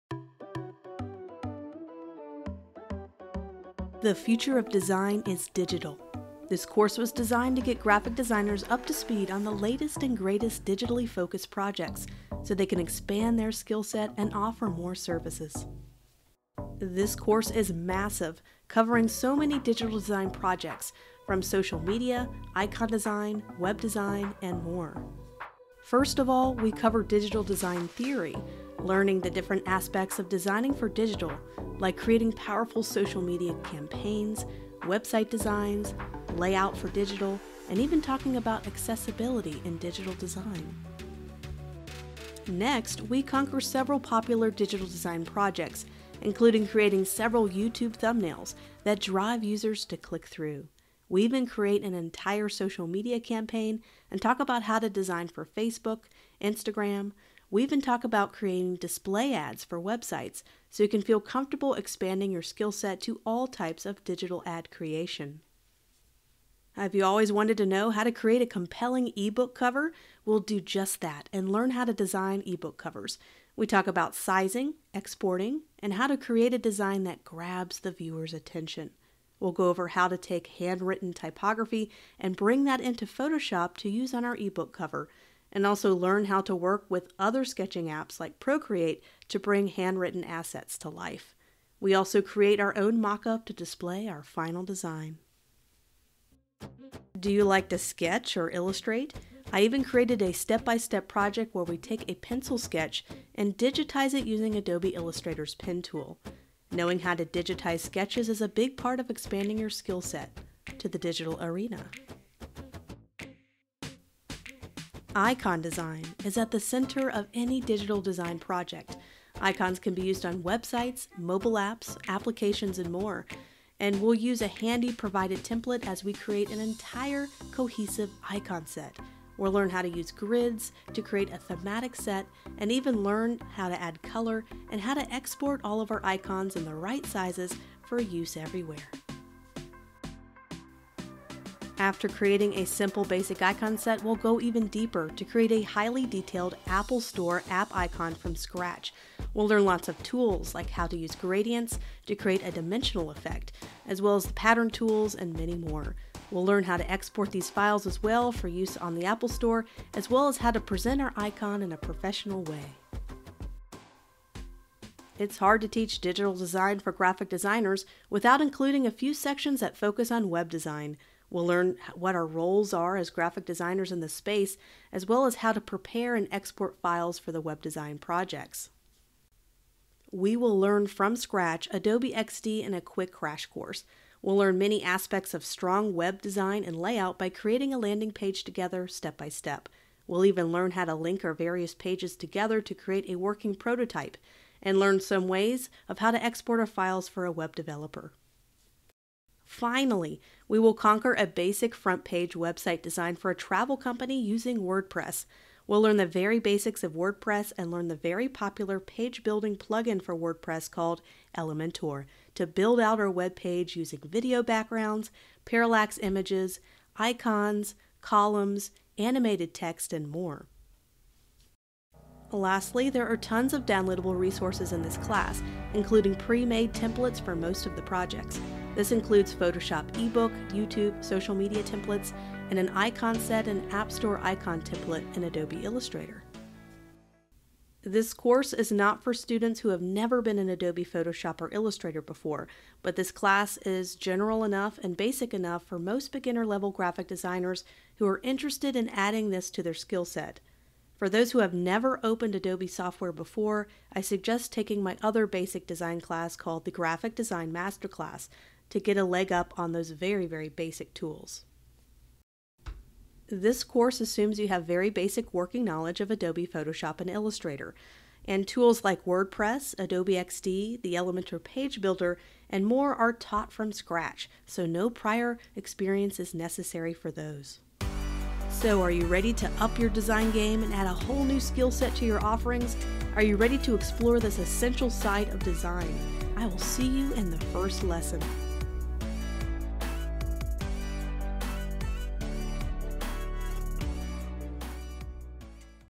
NOW LIVE! The Digital Design Masterclass for Graphic Designers
Now on BOTH Udemy and Skillshare - This 10+ hour course was designed for graphic designers wanting to learn and add new digitally focused design projects to their portfolios.

We cover the following topics and projects:
DIGITAL DESIGN THEORY
SOCIAL MEDIA DESIGN (Youtube, Facebook and Instagram)
ANIMATED GIFS
DIGITIZING SKETCHES
CREATING DIGITAL BRAND ASSETS
ICON SET DESIGN
APPLE APP STORE ICON DESIGN
E-BOOK COVERS
WEB DESIGN USING WORDPRESS
LANDING PAGE DESIGN USING ADOBE XD